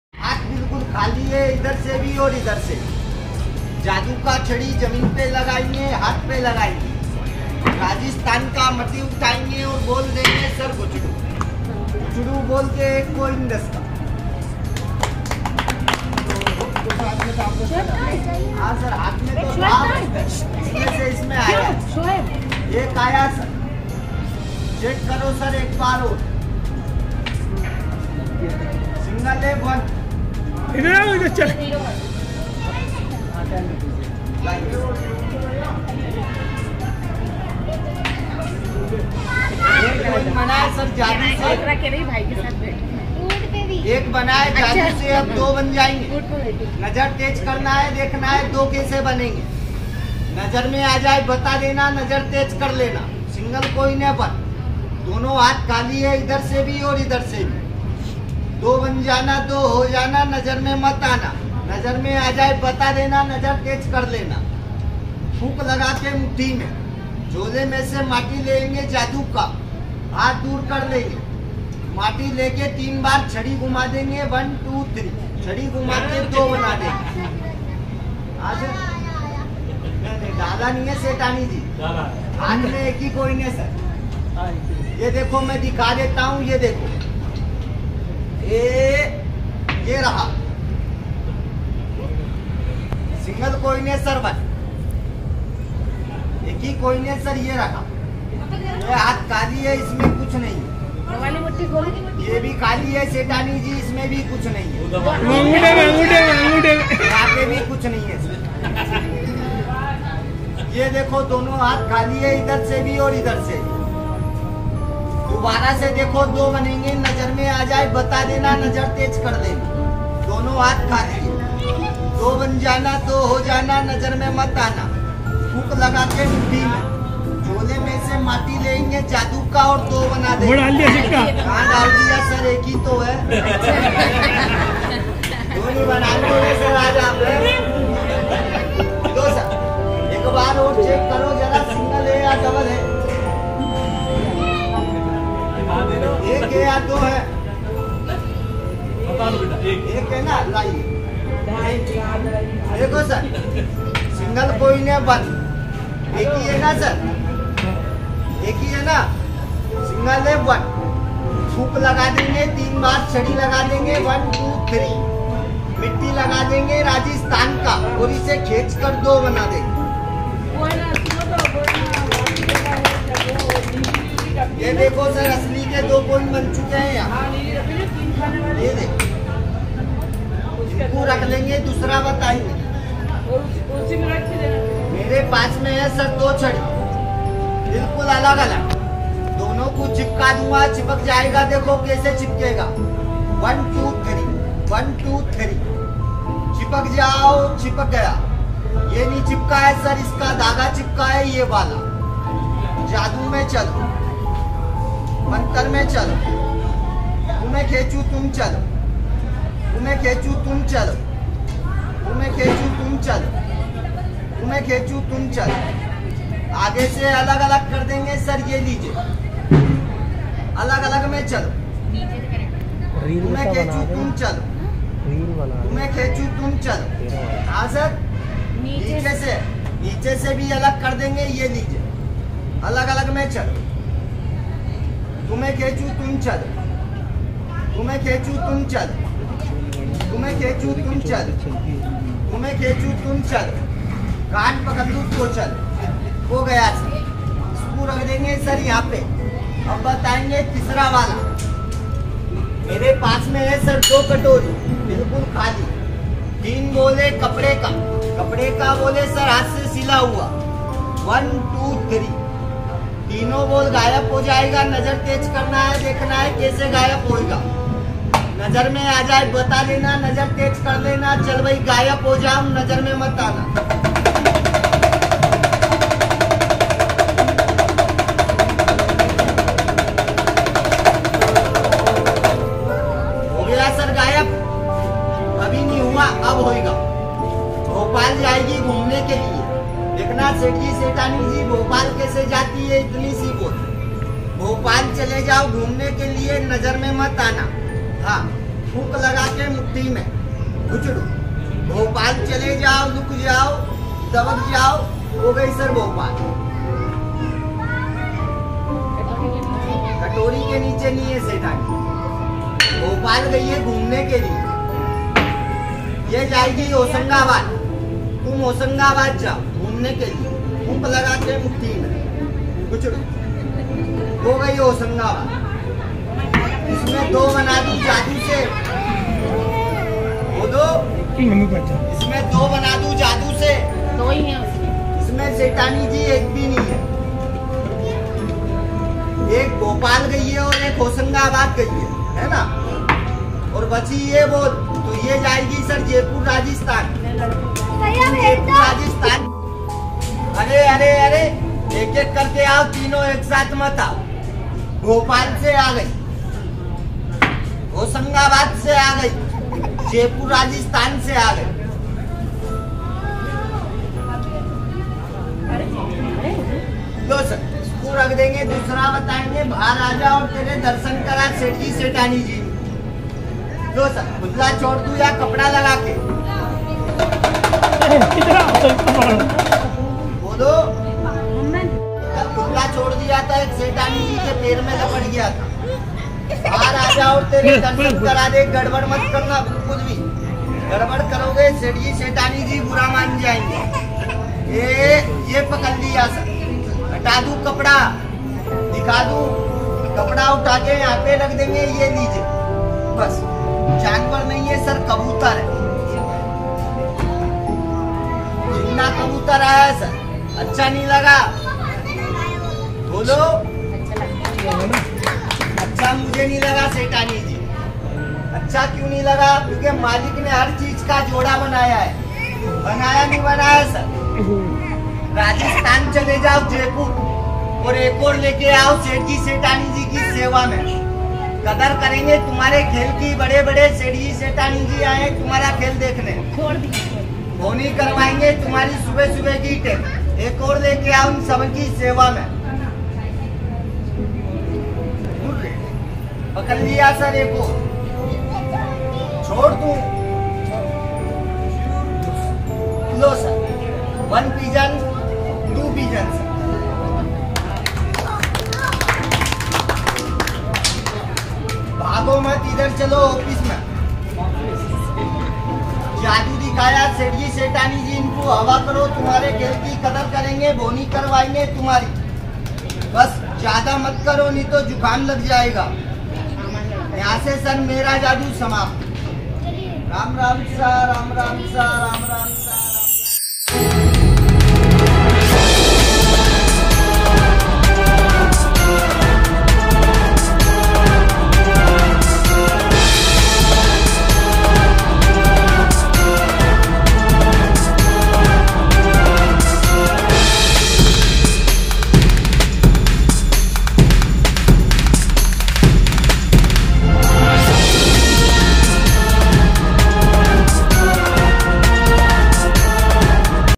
Indian local magician showing his skills.
This video shows a local magician showing his skills.